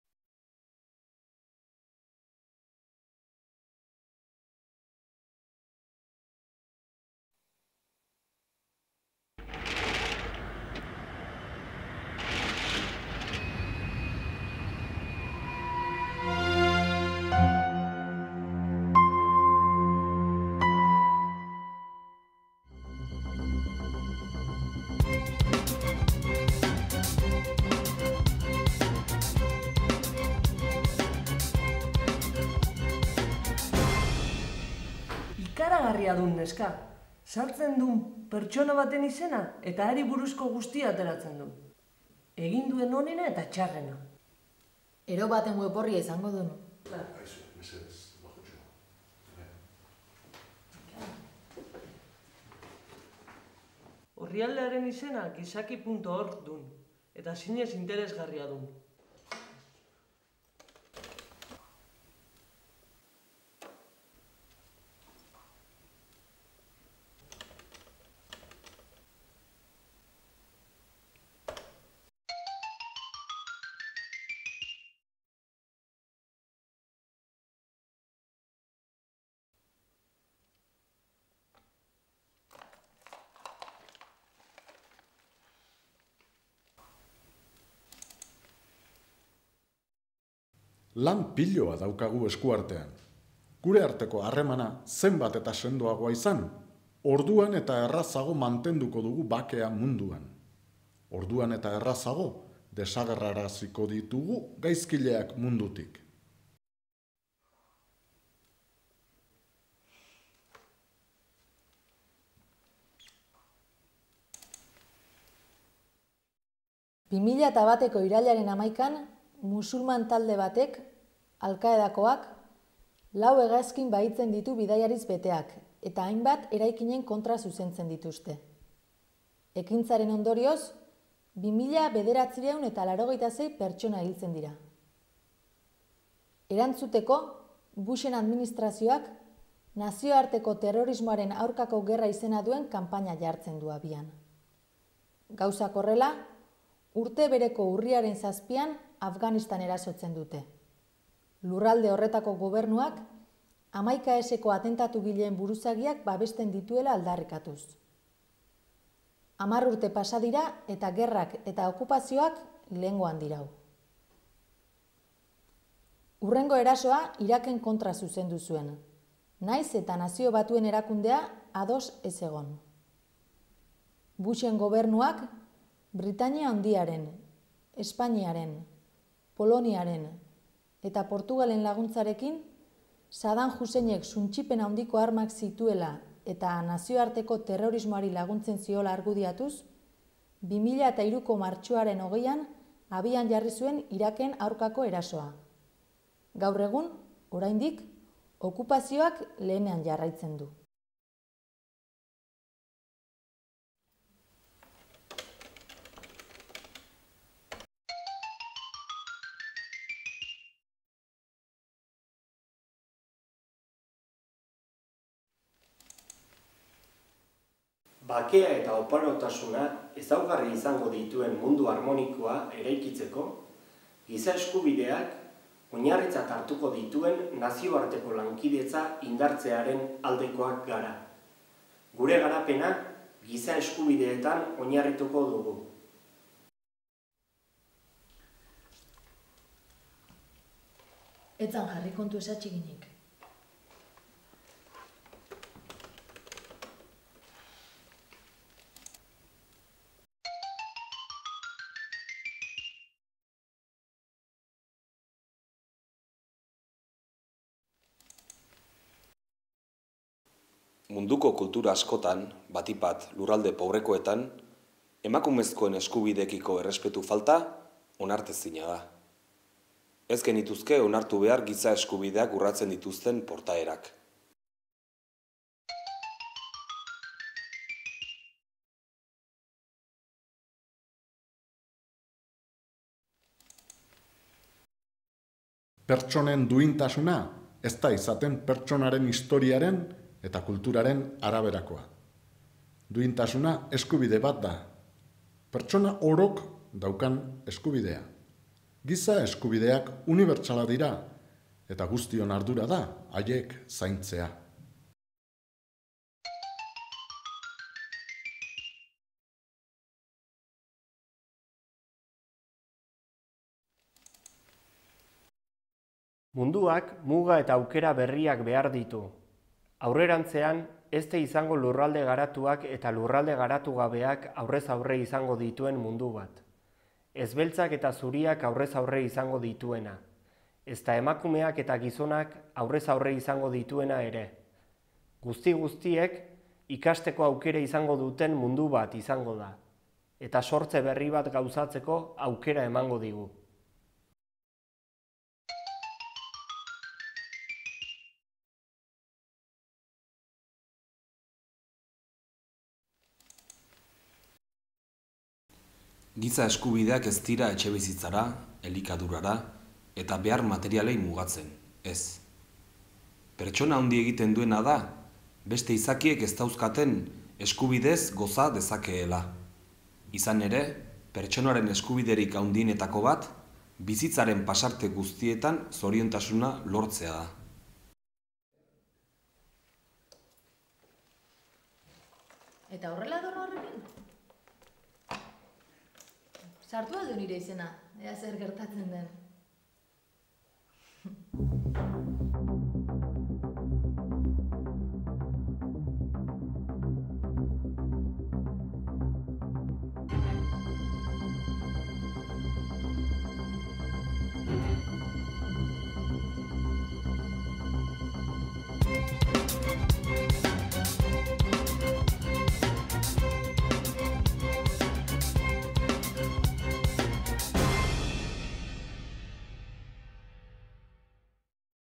GE -28- Giza Eskubideak: guztionak guztiontzat (onena eta txarrena)
Gaia: DBH - Herritartasunerako eta Giza Eskubideetarako Hezkuntza

Edukia eta erabilera:
Aldarrikapen Unibertsaleko 28. Atala oinarri hartuta ikus-entzunezko bat prestatu dugu ikasleen artean eztabaida eta iritzi trukea sustatzeko helburuaz.
Giza eskubideak ikuspuntu etiko eta moral batetik aztertzeak bizikidetzako oinarri moralak ulertzen laguntzen die ikaskideei, bai eta etika komun bat eraikitzeko zenbait teoria etikotatik hartzen diren zenbait elementu komunak identifikatzen ere, etika hori baita gizarte konplexu modernoetako bizikidetzaren oinarri.